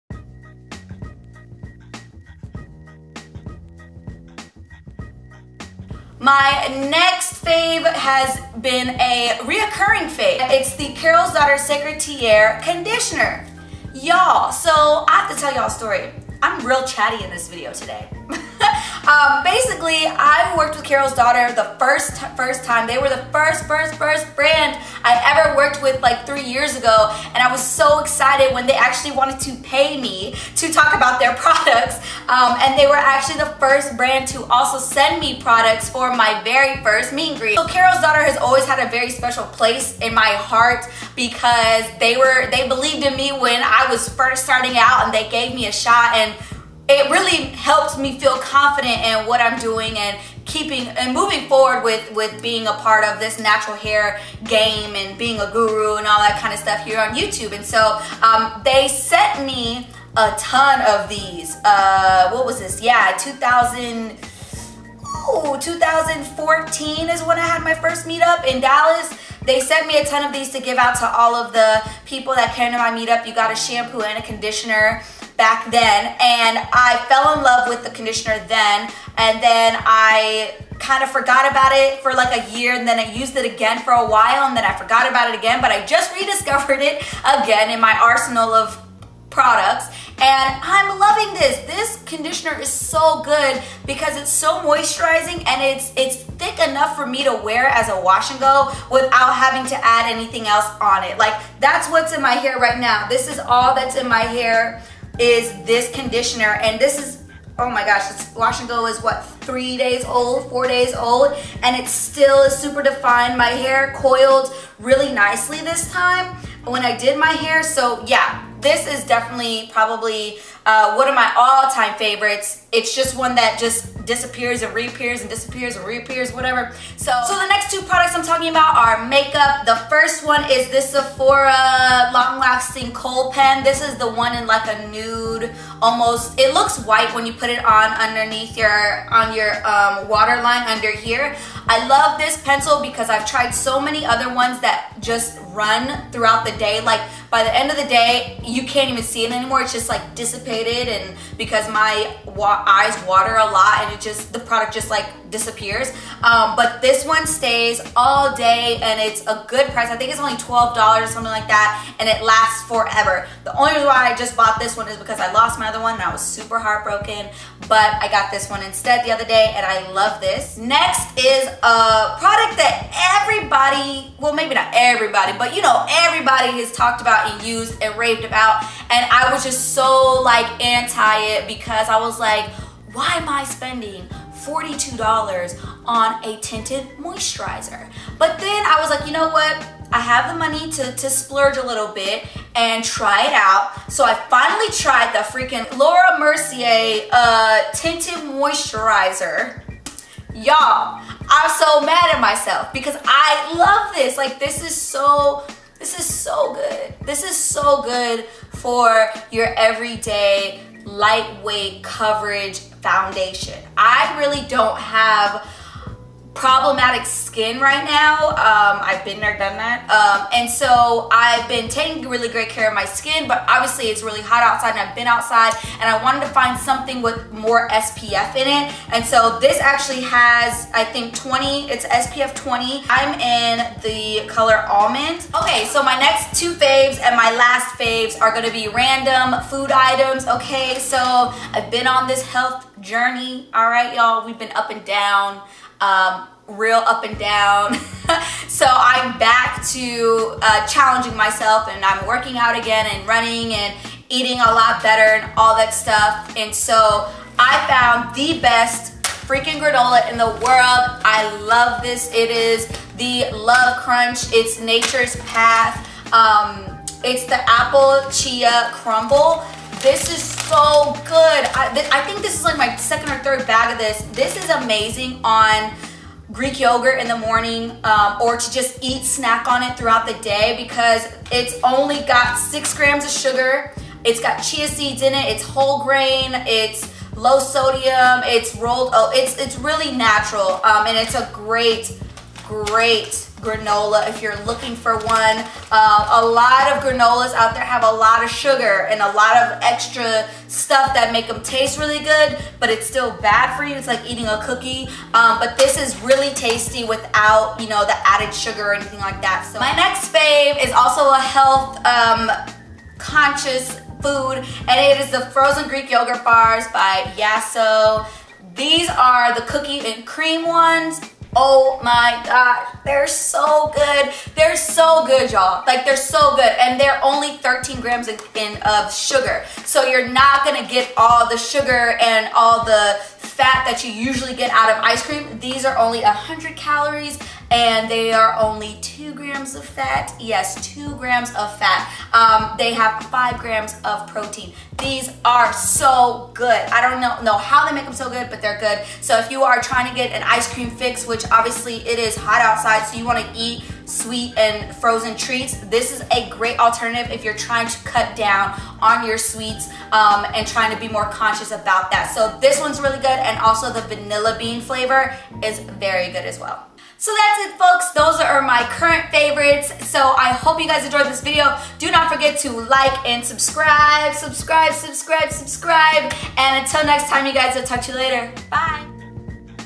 Current Favorites! PART TWO
Hey y'all! Here are my current favorite products! This video got pretty long so it is split up in two parts! This is part 2! No particular order in products just sharing what I am loving!

► W A T C H M Y L A S T V I D E O →

► @Lipstickncurls on Twitter

► @__LipsticknCurlss__ on Instagram

► @itsjadekendle on Snapchat
Commonly Asked Questions:
What do I use to edit film?: